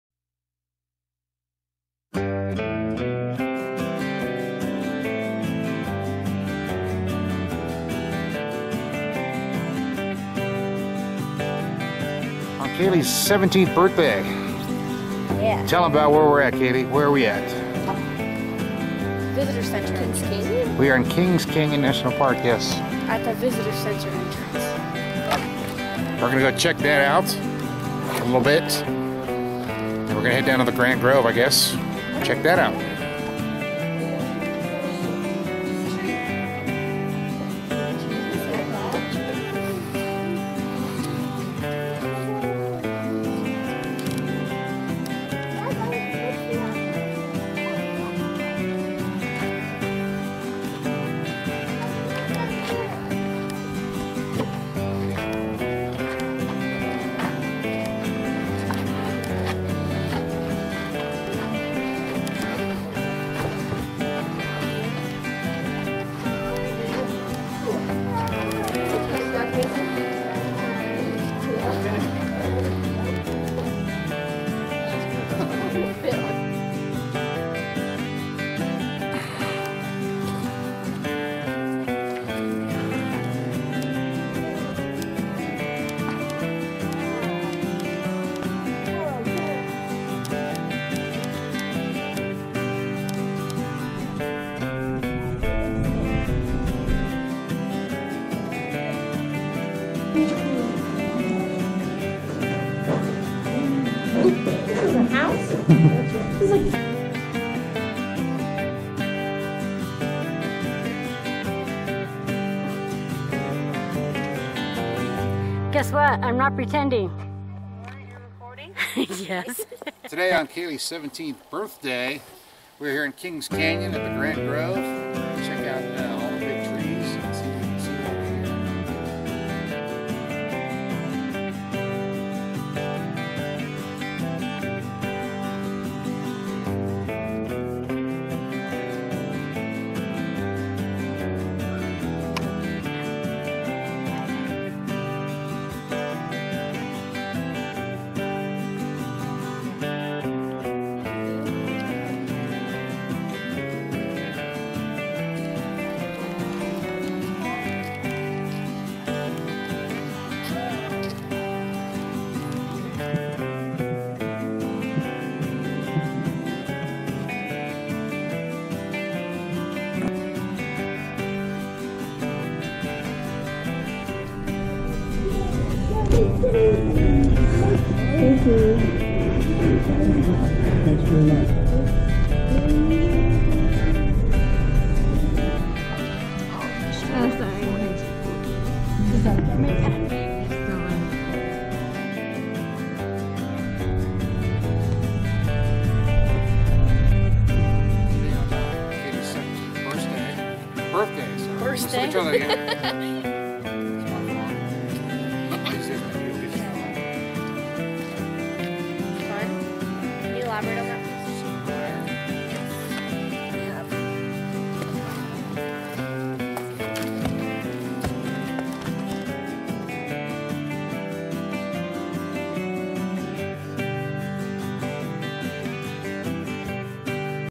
Kings Canyon Nat'l Park Big Trees' in the Fall Ep 1
Our Fall 2019 stop at Grant Grove in Kings Canyon National Park.